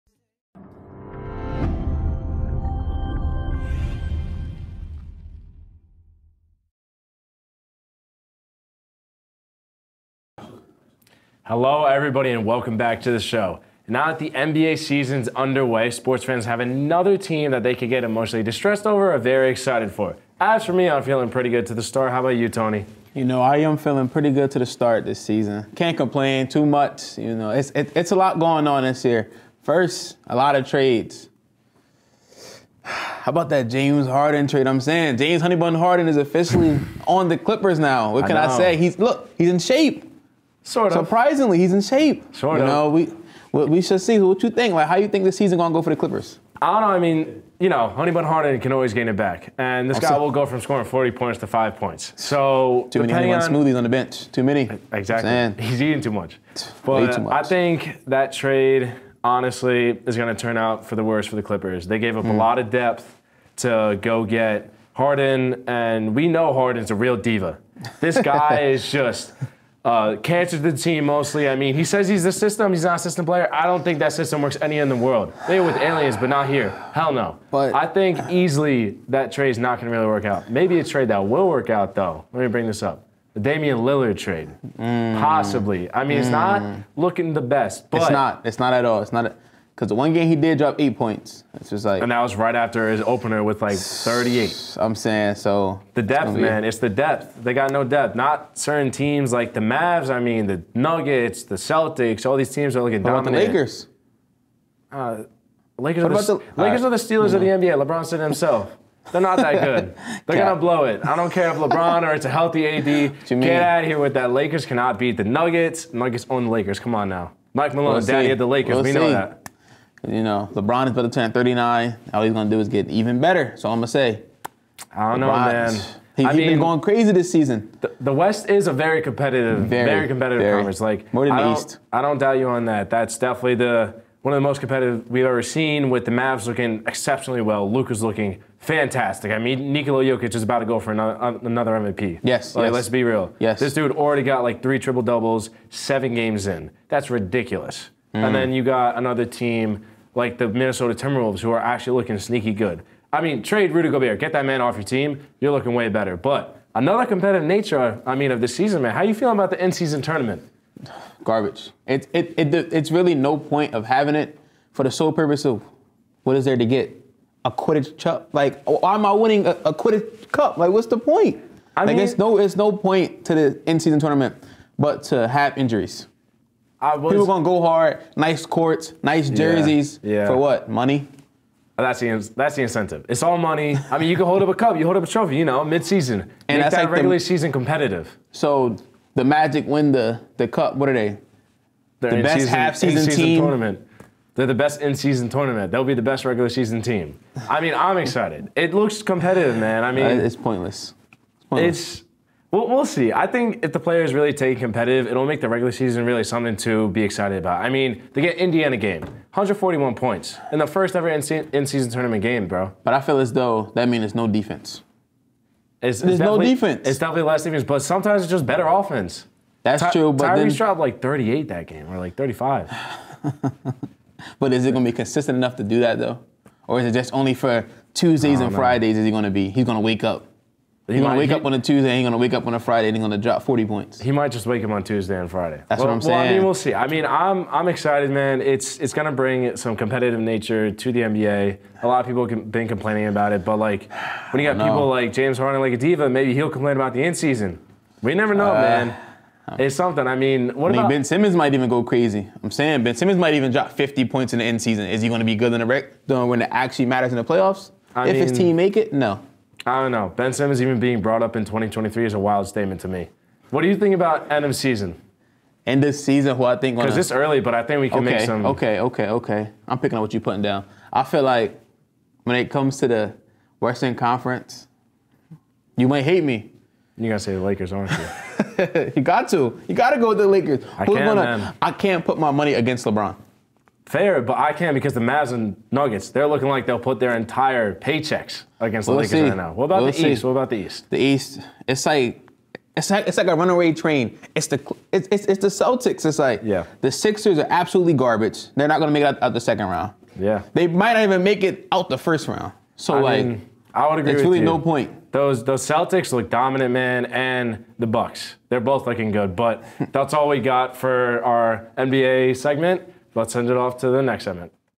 NBA Hot Takes with Tony and Rocco
Tony Dean and Rocco Bueti give us their Hot Takes on the NBA.